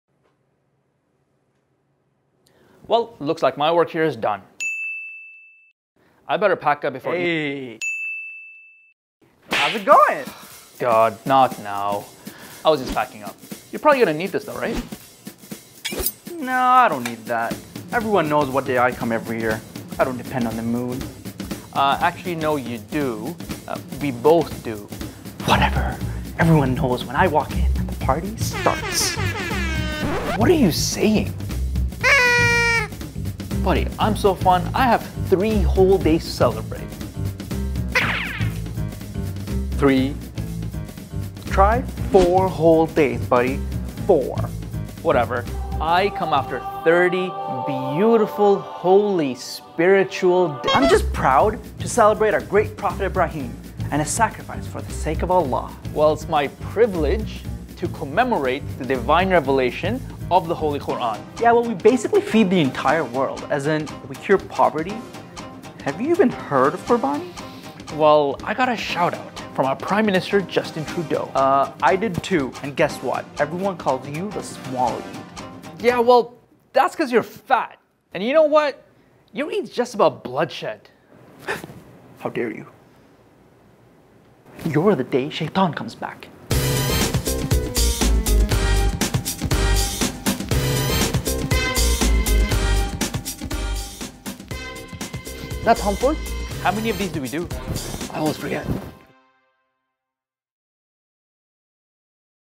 The Bigger Eid | Eid Mubarak!
We present to you: Eid al-Fitr and Eid al-Adha. The "little" Eid and the "big" Eid.

What happens when these two days come face-to-face? Would there be a little friendly competition?

We hope to insert a little humor in your day as we imagine a face-off between Eid al-Fitr and Eid al-Adha! After this, you'll never forget which Eid is which.

Wishing everyone an early Eid Mubarak!